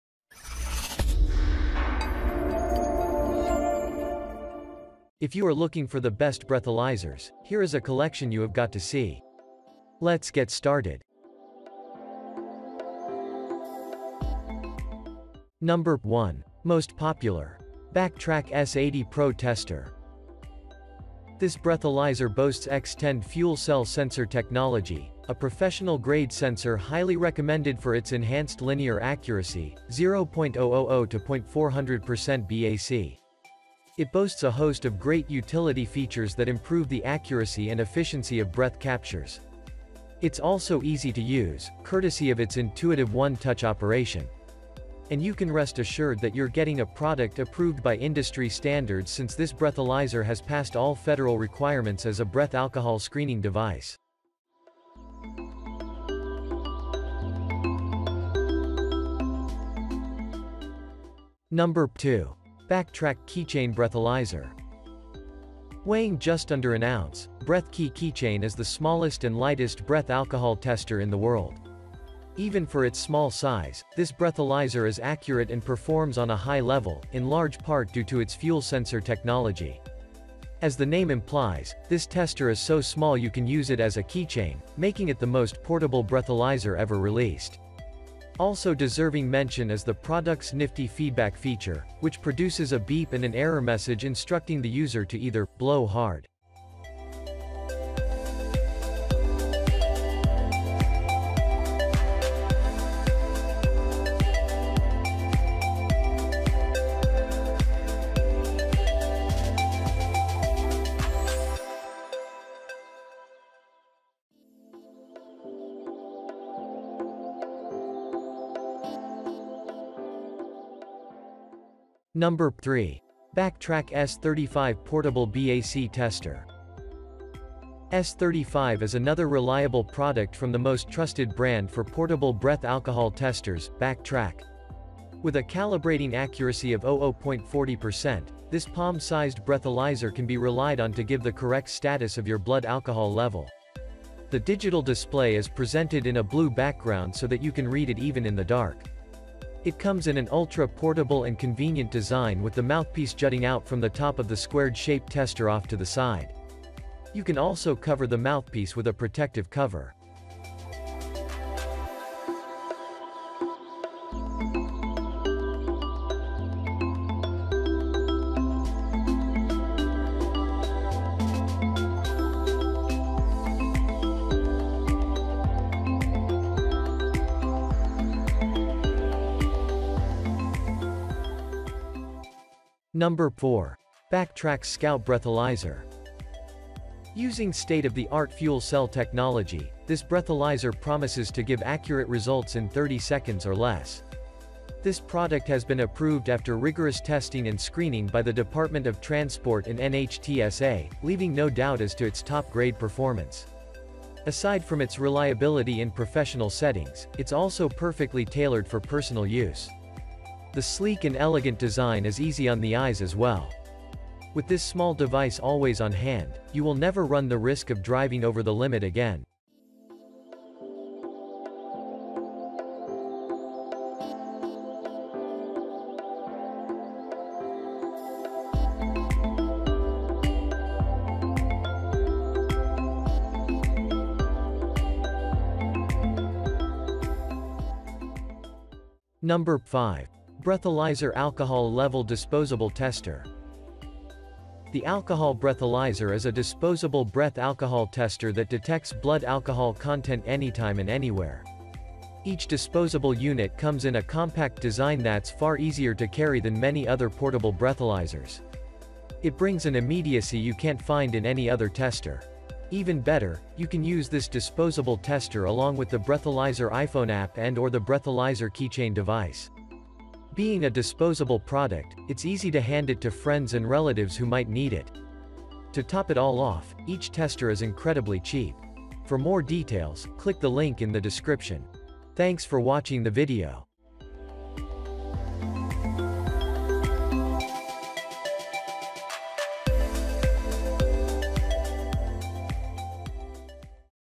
Top 5 Best Breathalyzers Review in 2023
Best breathalyzer's featured in this video:
0:13 NO.1. BACtrack S80 Pro Tester
1:01 NO.2. BACtrack Keychain Breathalyzer
2:03 NO.3. BACtrack S35 Portable BAC Tester
3:05 NO.4. BACtrack Scout Breathalyzer
4:00 NO.5. Breathalyzer-Alcohol Level Disposable Tester

What is a breathalyzer?
A breathalyzer is a device for estimating blood alcohol content (BAC) from a breath sample. The name is a genericized trademark of the Breathalyzer brand name of instruments developed by inventor Robert Frank Borkenstein in the 1950s.

What is the best breathalyzer?
When it comes to buying the right products, there's a lot to look out for. that's why today we've picked and reviewed the five best models for you. we've ranked them by many different factors such as price, quality, durability, performance, and more.